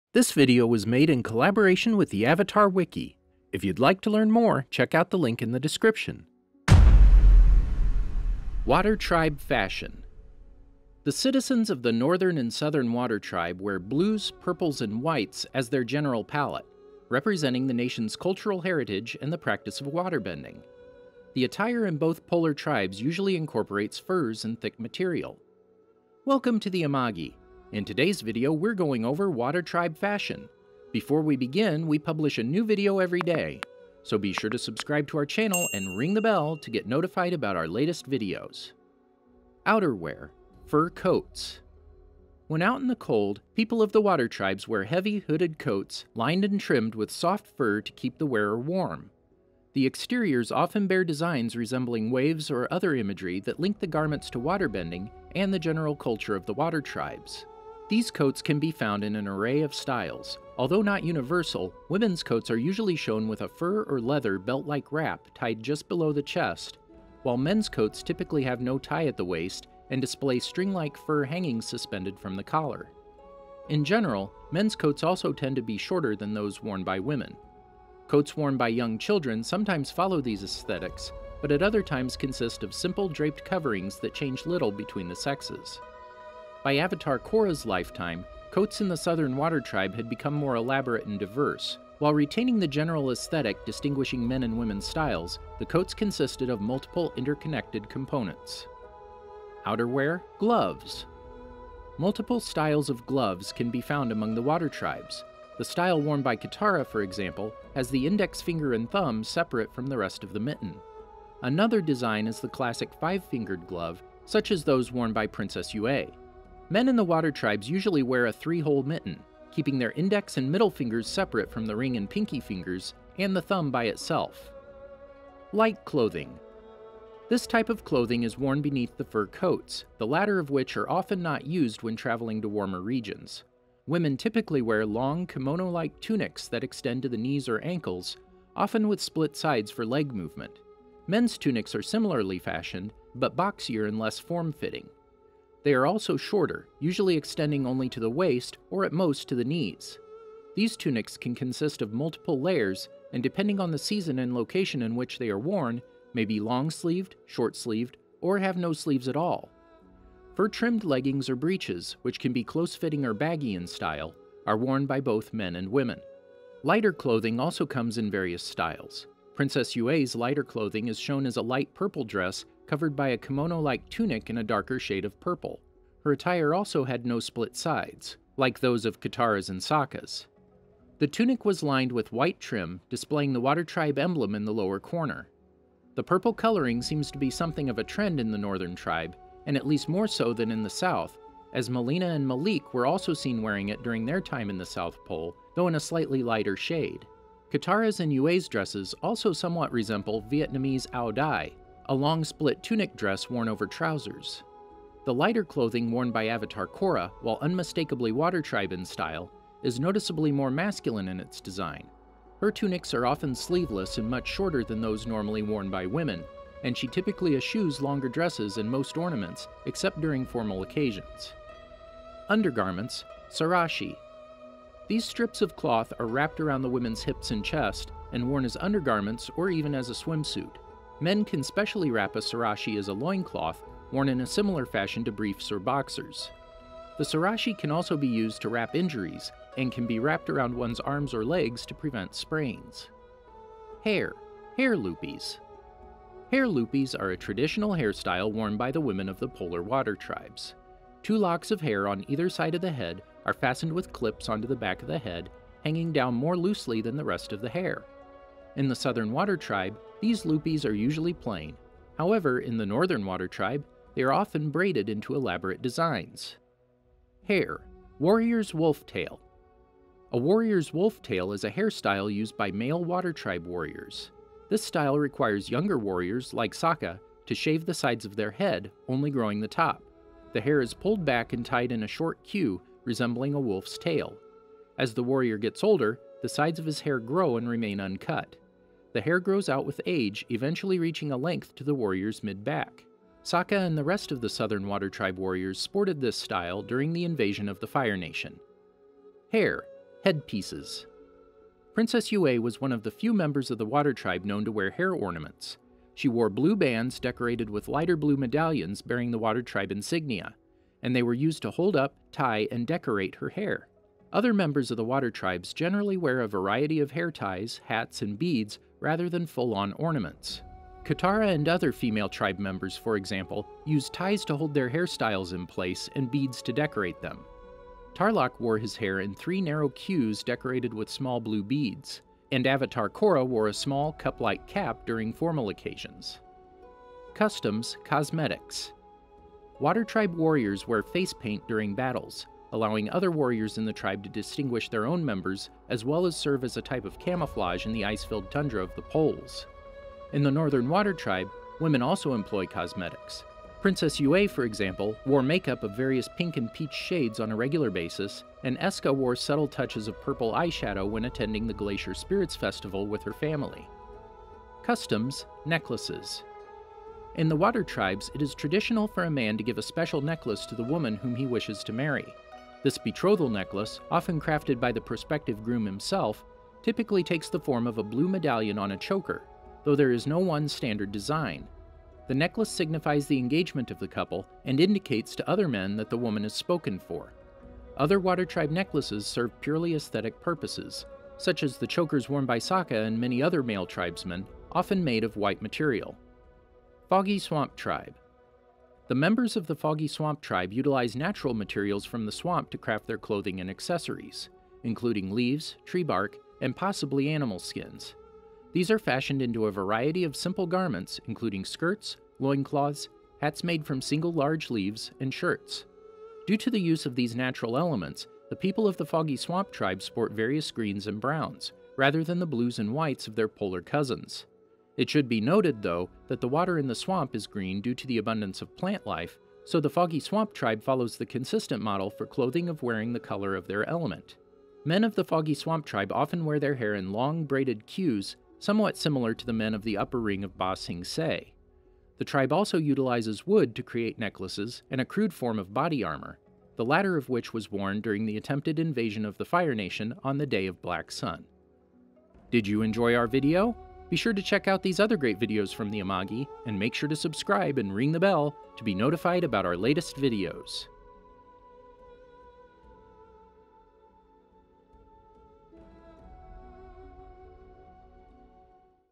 Water Tribe Fashion (Avatar)
The citizens of the Northern and Southern Water Tribe wear blues, purples, and whites as their general palette, representing the nation's cultural heritage and the practice of waterbending. The attire in both the polar tribes usually incorporates furs and thick material.

CHAPTERS

00:00 Intro
00:39 Fur Coats
01:39 Gloves
02:05 Light Clothing
03:42 Sarashi
04:03 Loopies
04:27 Warrior's Wolf Tail
04:59 Headpieces
05:37 Cosmetics
06:05 Necklaces
06:36 Foggy Swamp Tribe

AVATAR WIKI

CREDITS

PRODUCER: @AmagiPublishing
NARRATOR: @Jeremyafreed
EDITOR: @Imubi5
THUMBNAIL: @MackyDraws